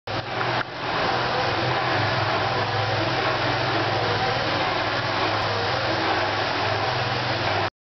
Tunel strazevica u izgradnji
Zavrsni radovi u Tunelu strazevica